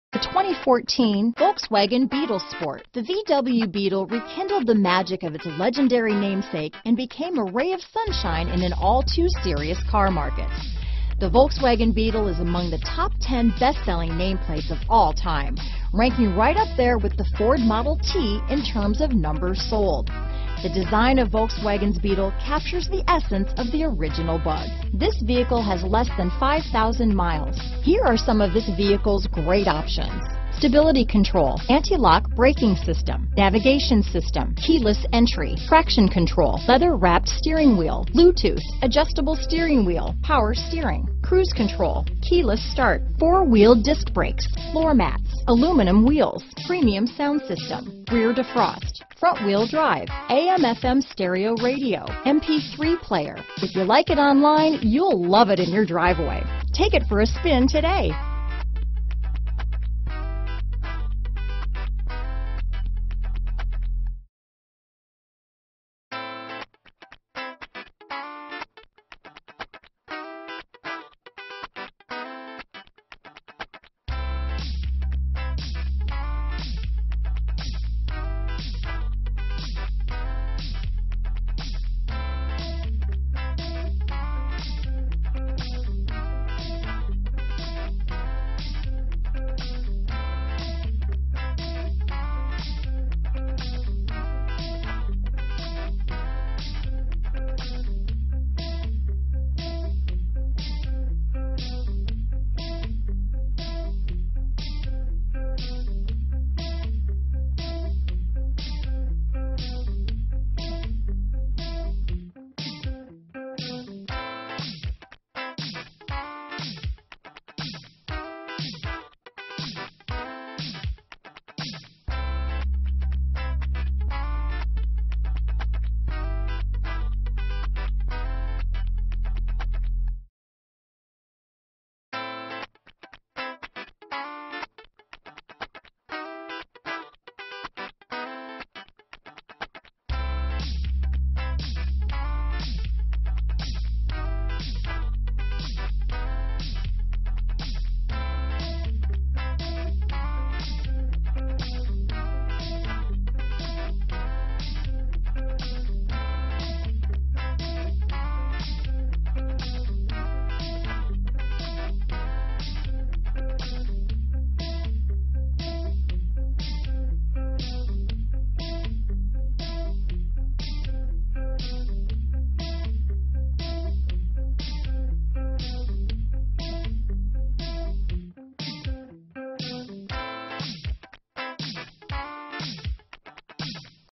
2014 Volkswagen Beetle Convertible Carson City, Reno, Northern Nevada, Dayton, Lake Tahoe, NV 14038
2014 Volkswagen Beetle Convertible
2014 Volkswagen Beetle Convertible 2.5L w/Sound/Nav - Stock#: 140382 - VIN#: 3VW5P7AT5EM805786

Come see this 2014 Volkswagen Beetle Convertible 2.5L w/Sound/Nav. Its Automatic transmission and Regular Unleaded I-5 2.5 L/151 engine will keep you going. This Volkswagen Beetle Convertible features the following options: Wireless Streaming, Window Grid Diversity Antenna, Wheel: 18 Alloy, V-Tex leatherette seating surfaces, Variable Intermittent Wipers w/Heated Jets, Valet Function, Urethane Gear Shift Knob, Trip Computer, Transmission: 6-Speed Automatic, and Transmission w/Driver Selectable Mode and Sequential Shift Control. Test drive this vehicle at Carson City Toyota, 2590 S Carson St, Carson City, NV 89701.